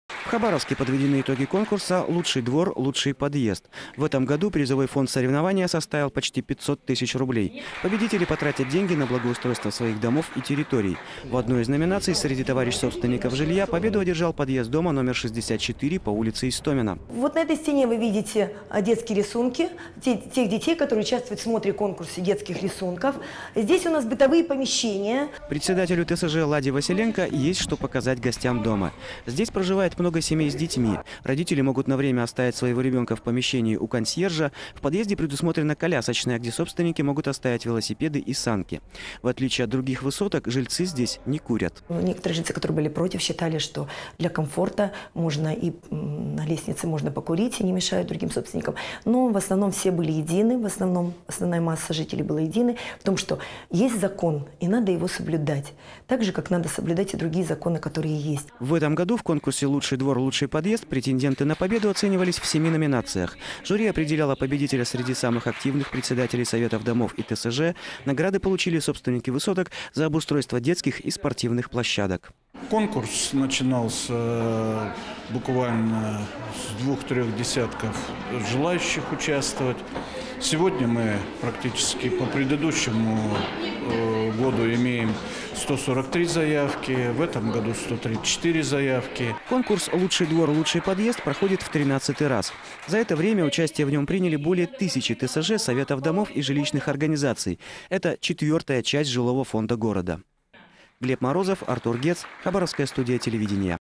Лучший подъезд
В Хабаровске подведены итоги конкурса «Лучший двор, лучший подъезд» В материале возможно упоминание платформ корпорации Meta (запрещена на территории Российской Федерации)